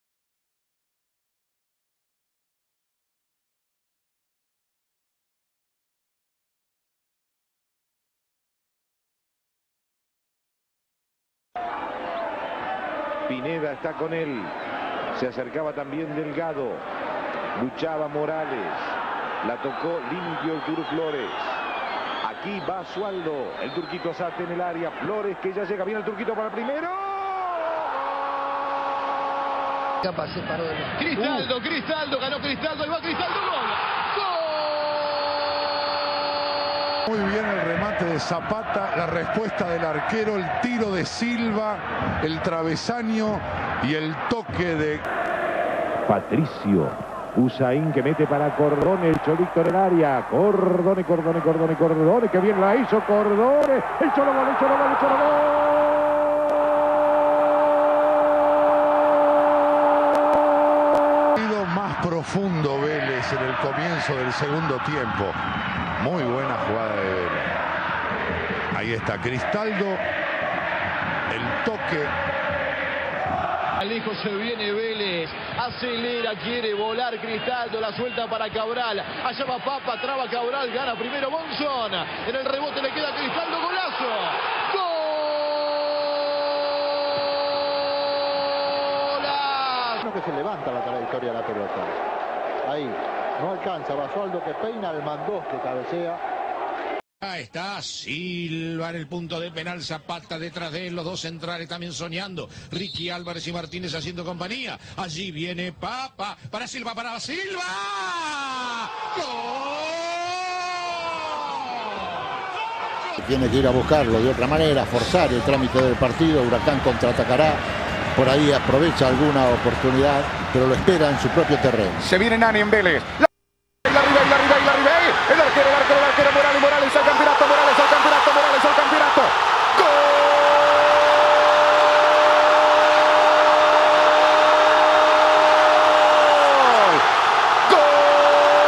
[PFRetro] Vélez - Huracán | Previa
En un nuevo PFRetro revivimos los mejores goles en la previa de Vélez - Huracán